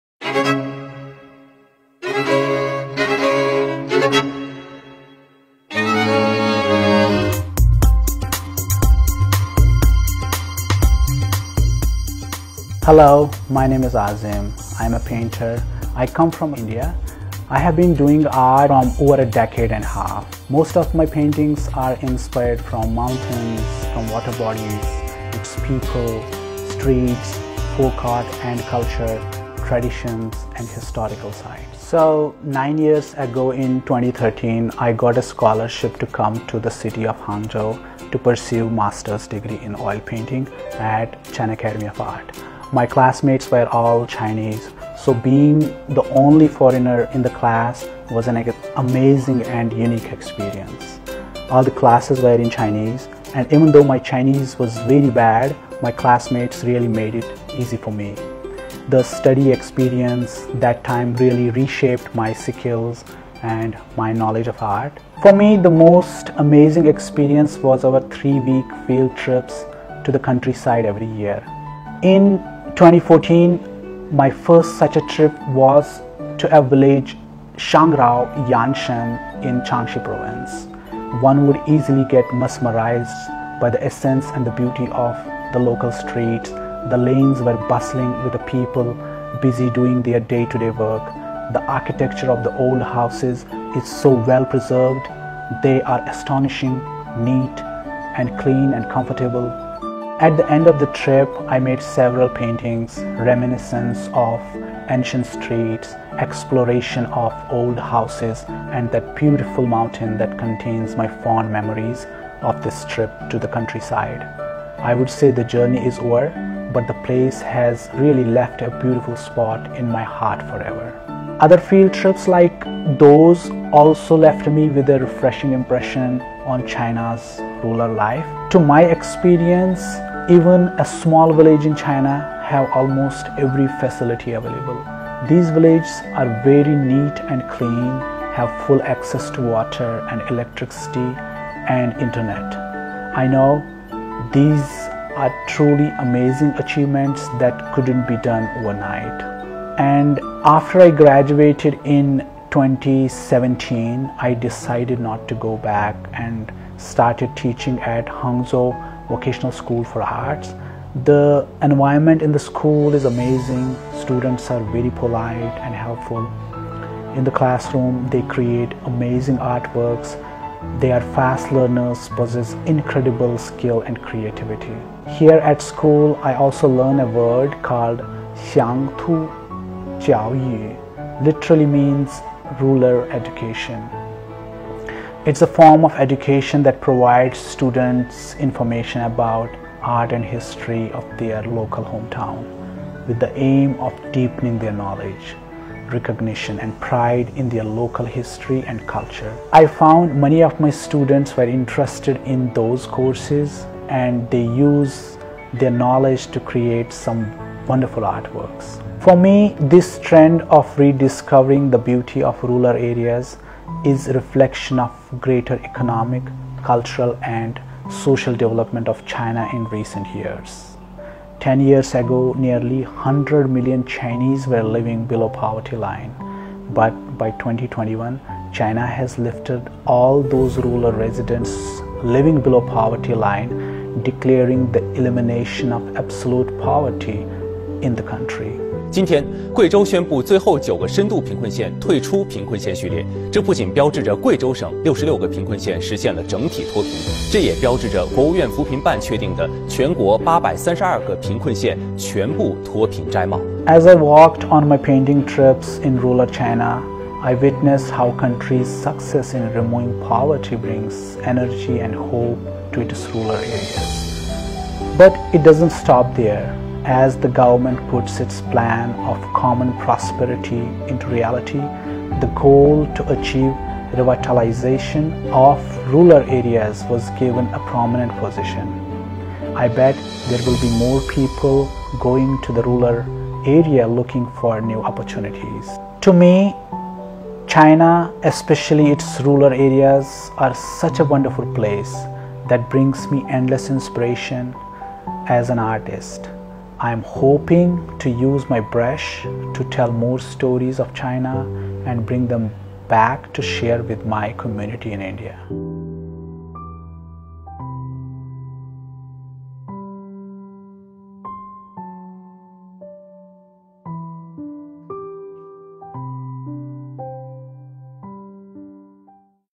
【大美中國！印度畫家畫筆下的中國鄉村】來自印度的畫家Azim將中國鄉村當作了“靈感繆斯”，他寄情於山，托情于水，創造出很多精彩的作品：江南小鎮、自然光影、鄉村小巷……一起看看他筆下的中國鄉村之美吧！
中國讓我沒想到# 提起油畫藝術，你第一個想到的作品是什麼？ 來自印度的畫家Azim，將中國鄉村當作了“靈感繆斯”，他寄情於山，托情于水，創造出了很多精彩的作品：江南小鎮、自然光影、鄉村小巷……一起看看他筆下的中國鄉村之美吧！